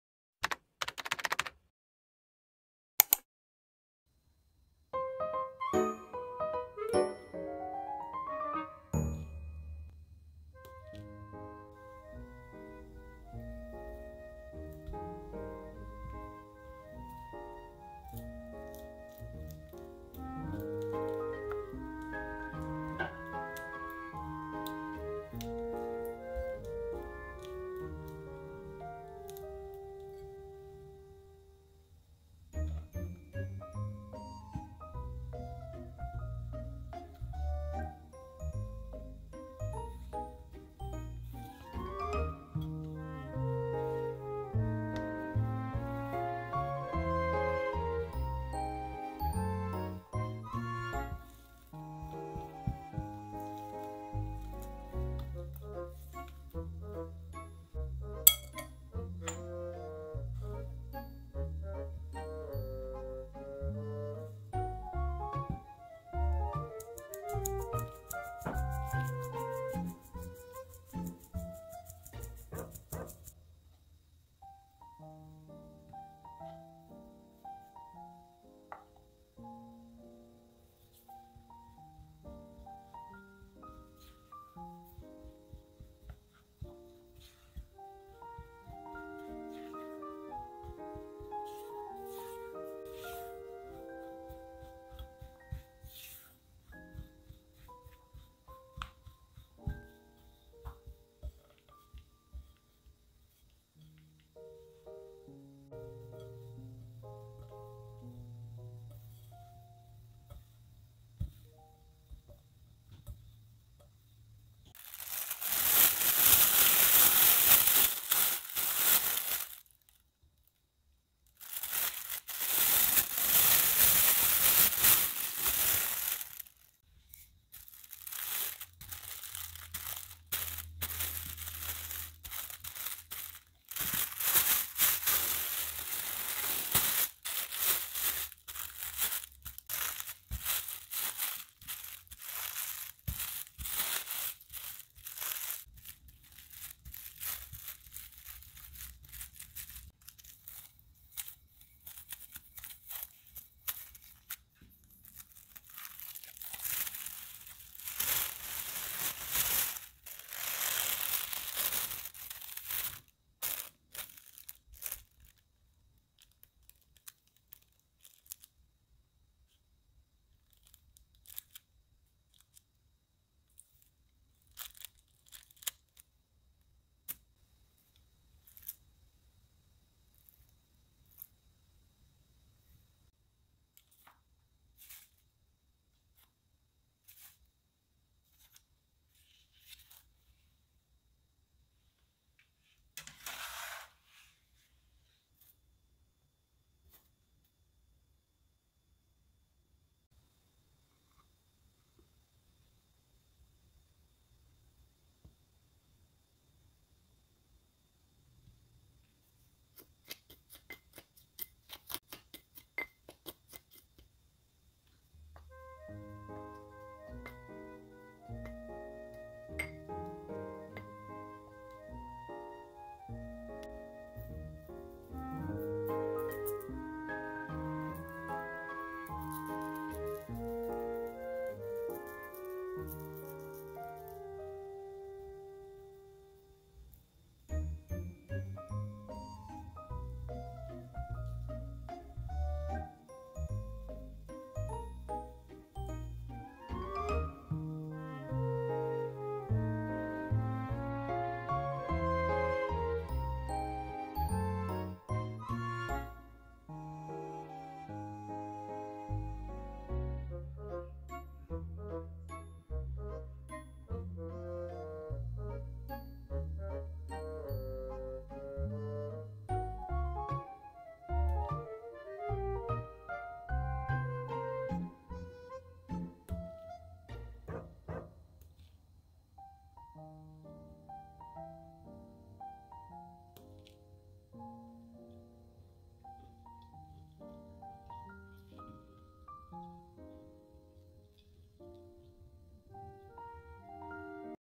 BLOSSOM COOKIES 🌸🌼🌸🌼🌸| NO EGG Easy Cookies
How to make quick and easy snow or blossom cookies that melt in your mouth.

Hi, welcome to my Let`s Stella channel.

Ingredients ( 4cm - 20 cookies )
60g butter- room temperature
50g powdered sugar
1/4tsp vanilla extract
pinch of salt
90g cake flour
40g almond flour
For sprinkles
1.  20g powdered sugar
2.  10g strawberry powder, 10g powdered sugar
3.  13g matcha powder, 7g powdered sugar

🍪bake 300F for 13~14 minutes
🍪dough size 25 x 14cm
🍪cookie cutter size 4cm

Including thumbnail images or written recipes.